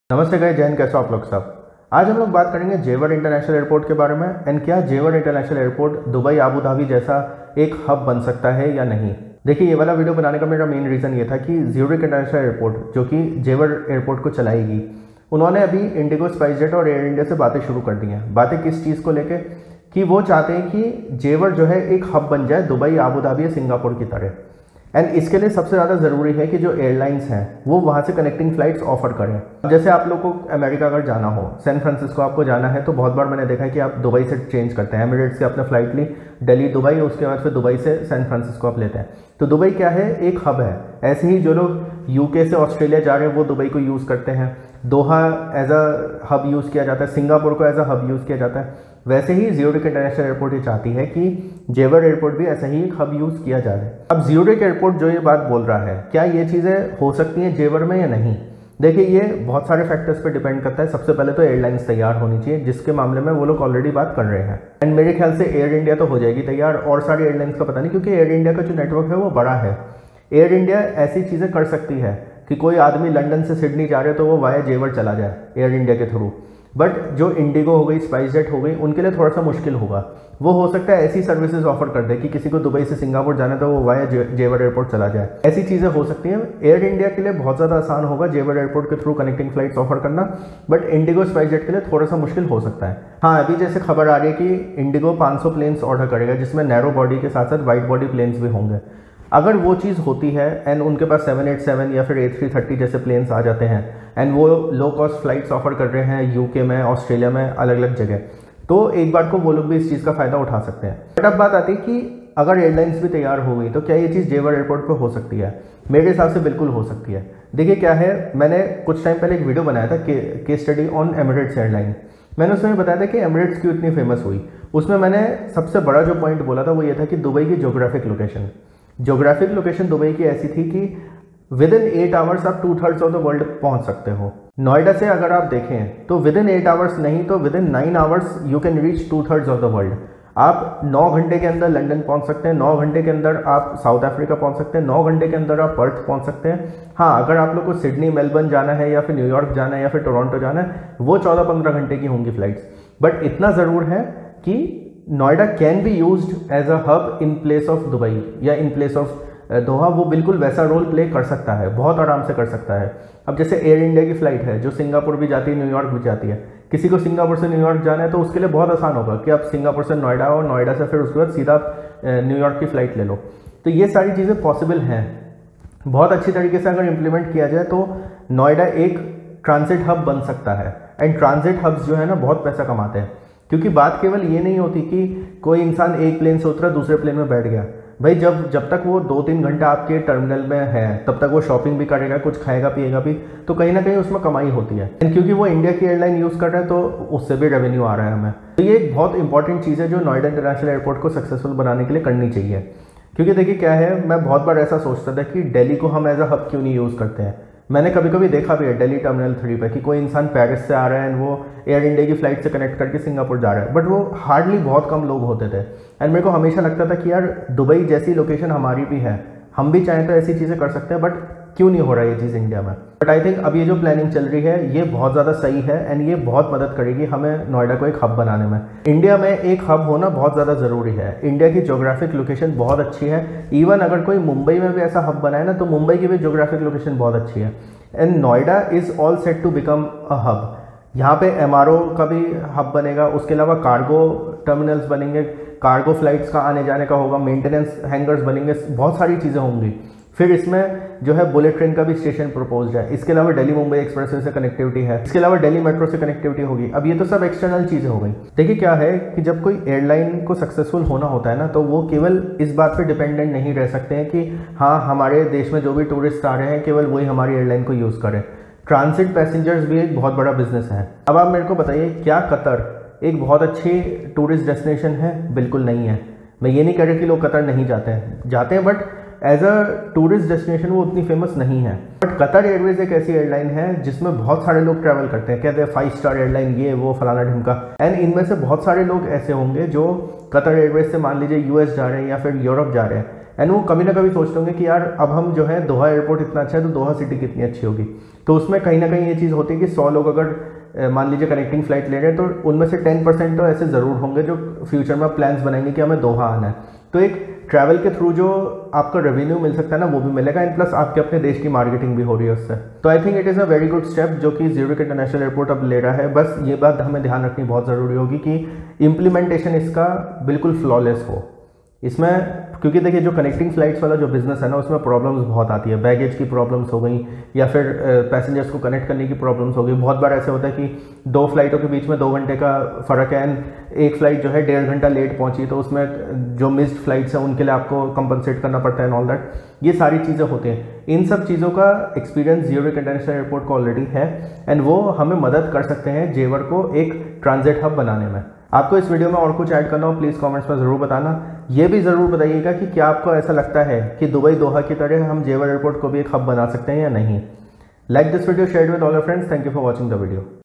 Can Jewar Airport become a Connecting Hub for International Passengers?
Zurich International Airport has the responsibility of operating Jewar Airport in Noida. Recently, they had talks with Indigo, Air India and Spicejet regarding connecting flights from Jewar. If this goes through, Jewar can become a connecting hub like Dubai or Singapore. In this Video, we have discussed the possibilities behind the same.

My New Book on Civil Aviation on AMAZON INDIA:

My New Book on Civil Aviation on AMAZON WORLDWIDE: